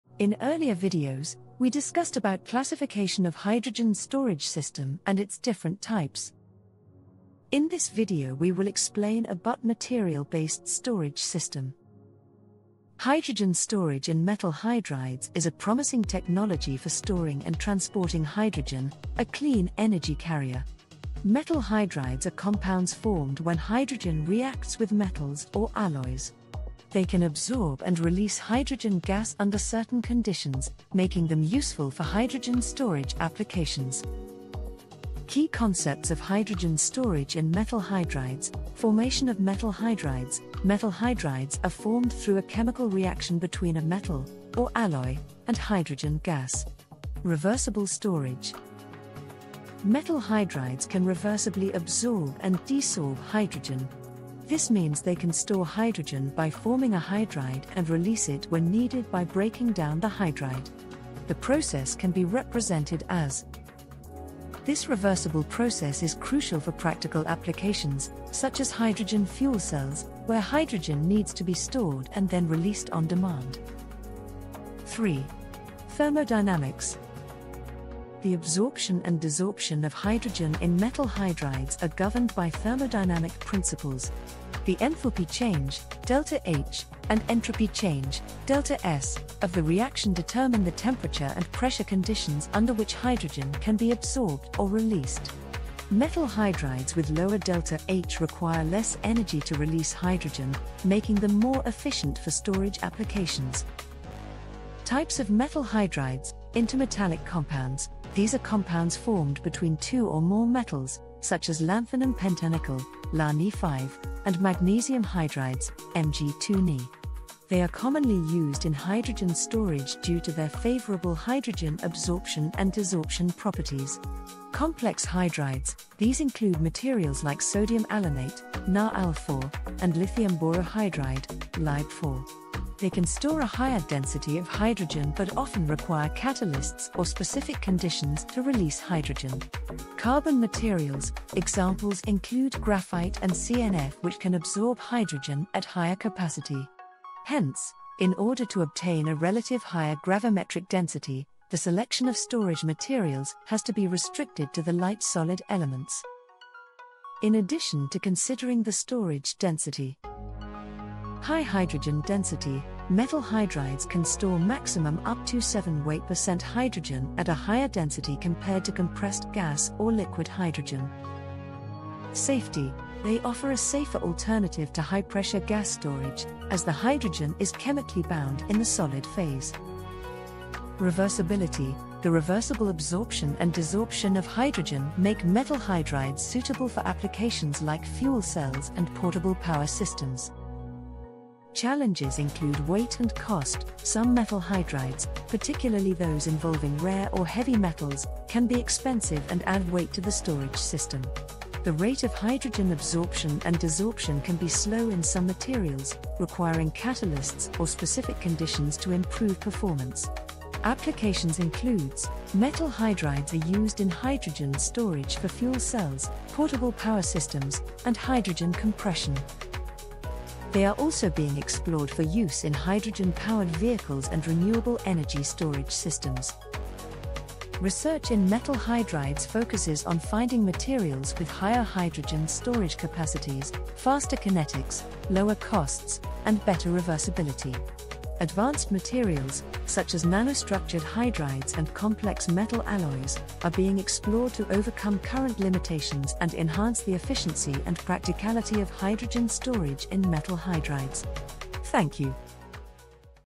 Hydrogen Storage System: Part-5 Metal Hydrides
In this video we will explain briefly about Liquid Hydrogen Metal Hydrides.